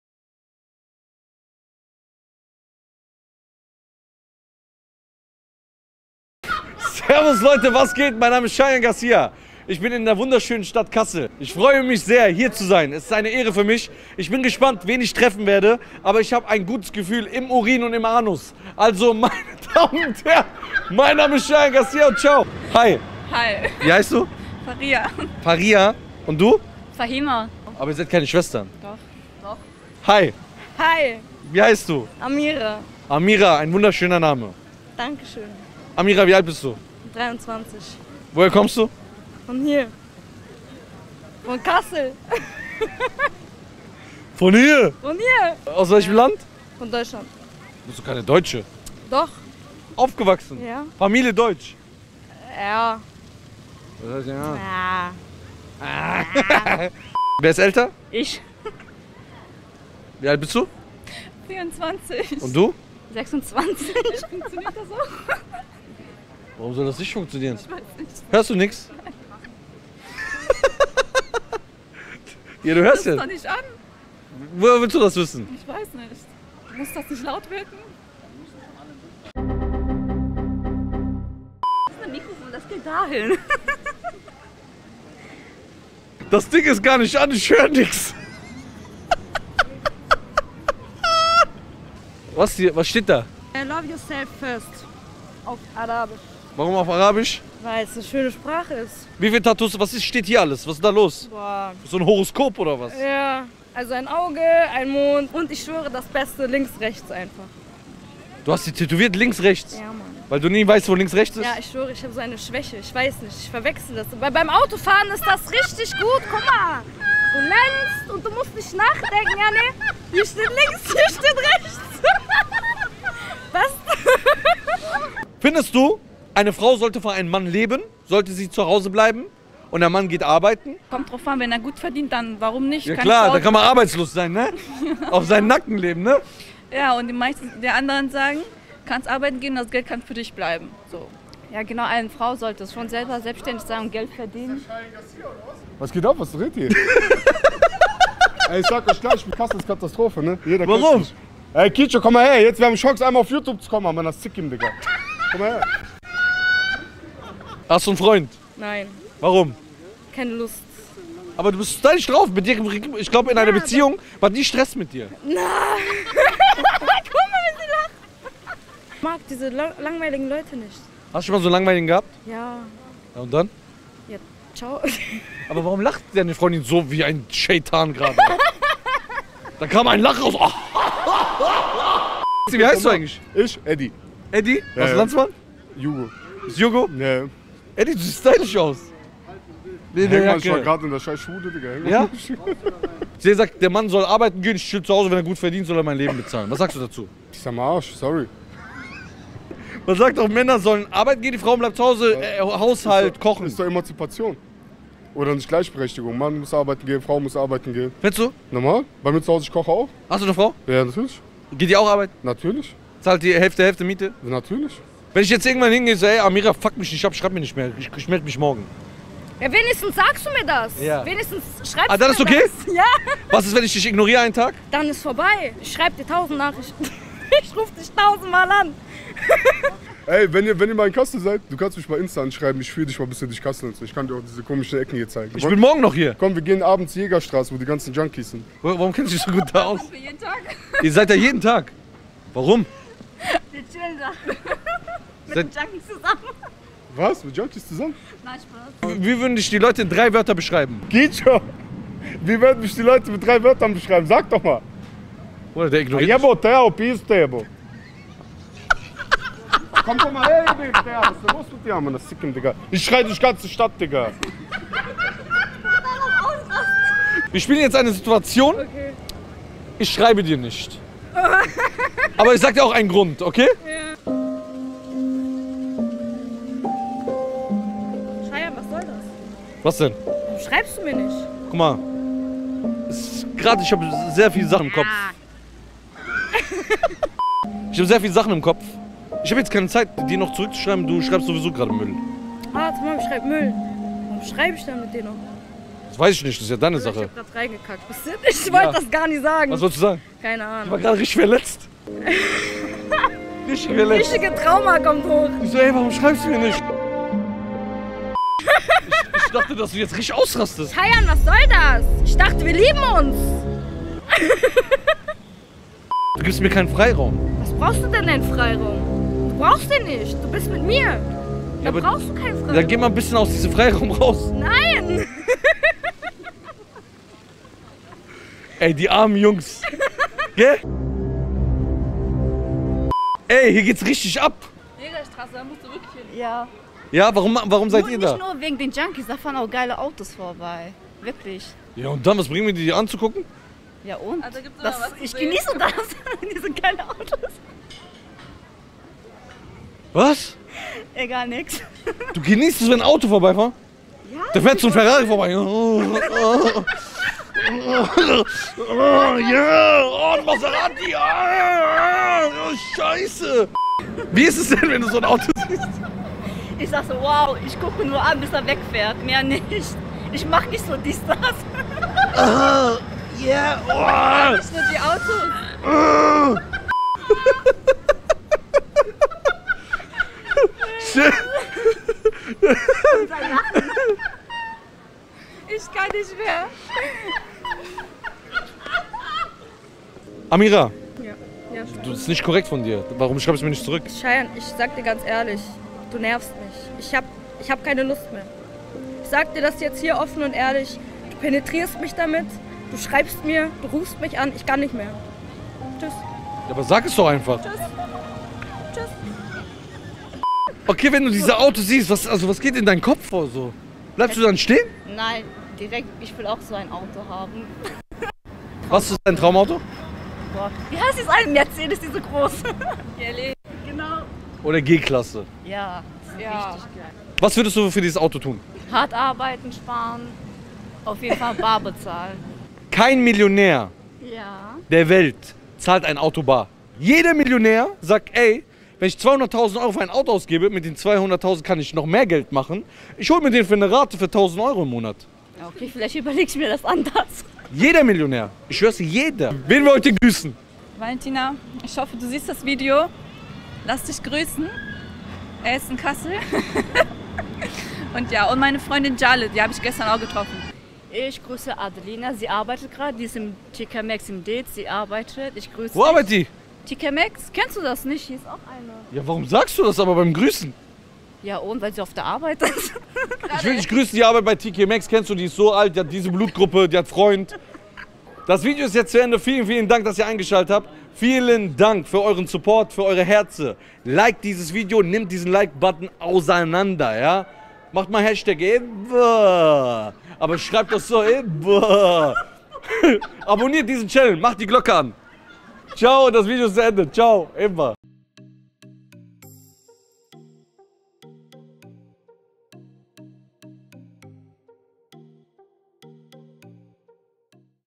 Sie steht auf fette Autos | Eifersüchtig | Shayan Garcia
Chillen in der Junkiestraße. Eifersüchtige Singles. Kassel hat wieder nur positiv verrückte Leute!